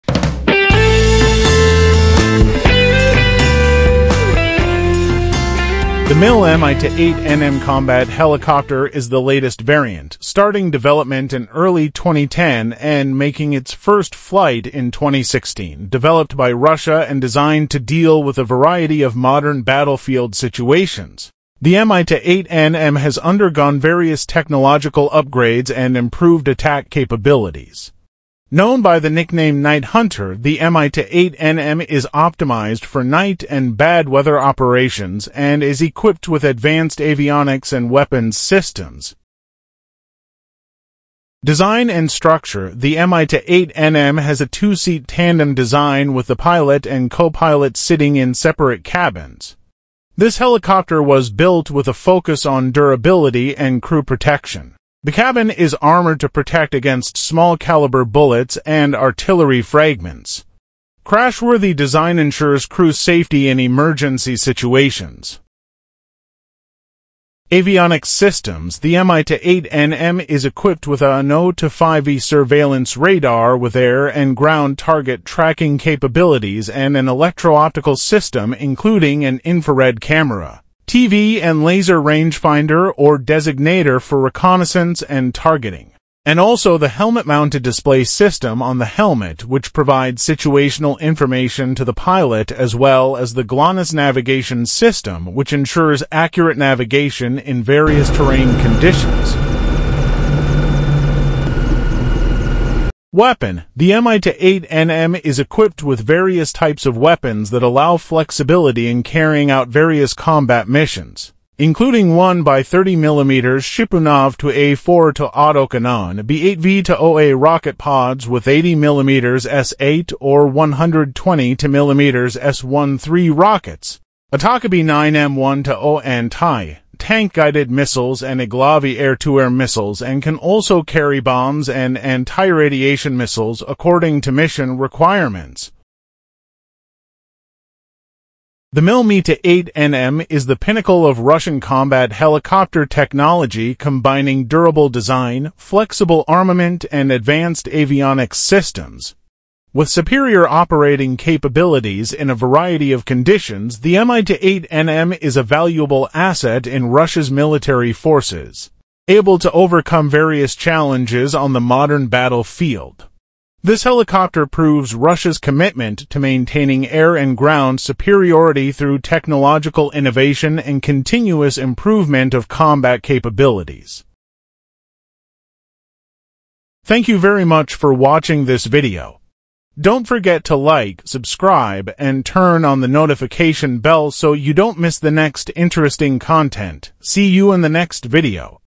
Mil Mi-28 NM Helicopter, The Night Hunter
The Mil Mi-28NM helicopter, also known as the "Night Hunter," is a Russian-made advanced attack helicopter designed to carry out day and night combat missions in all weather conditions. Equipped with modern avionics technology, advanced sensor systems and lethal weapons, the Mi-28NM is capable of destroying ground targets including armored vehicles, as well as air targets with high precision. This helicopter also has a high layer of protection for its crew, as well as extraordinary maneuverability that makes it one of the strongest and most reliable attack helicopters in the world today.